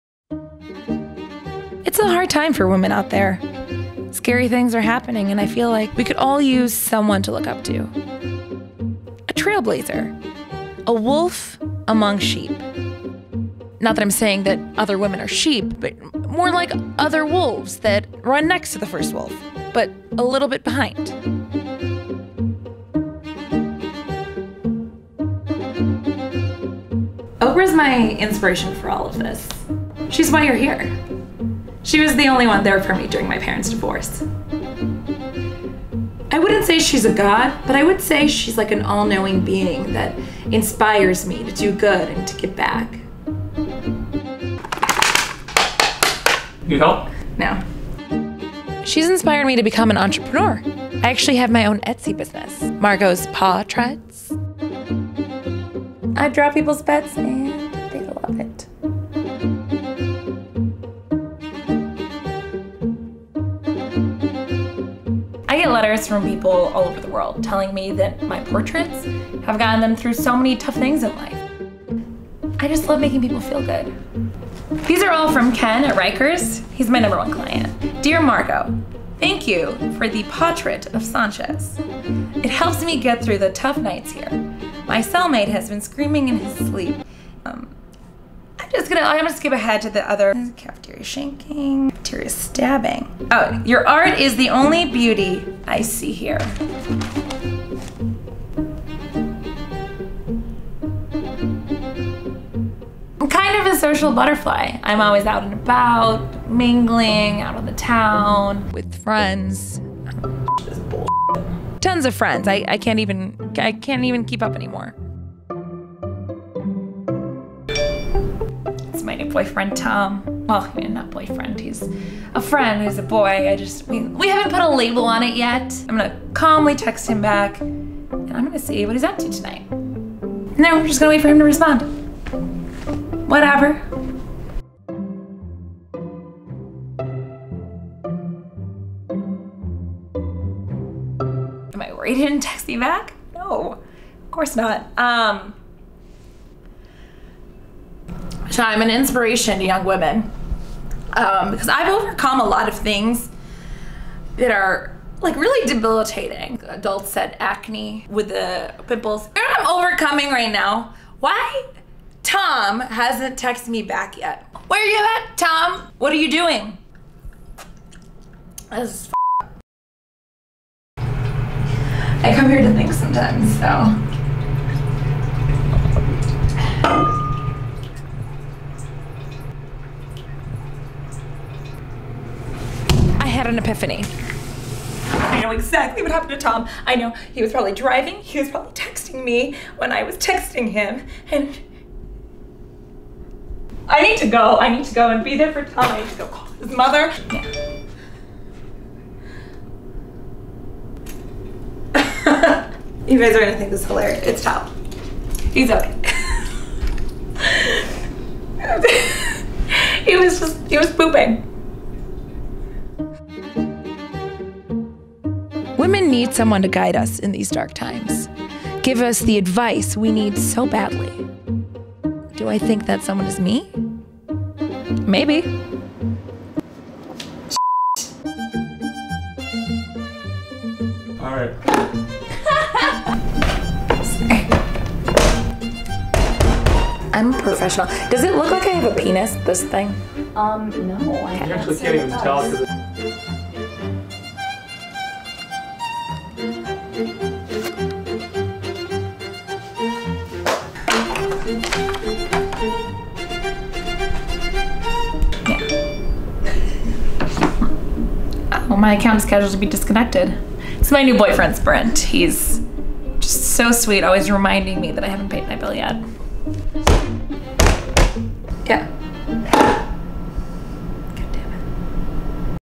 Advice to an American Woman (Pilot)
Entrepreneur, role model, and philanthropist Margot Smith shares her expertise with the world.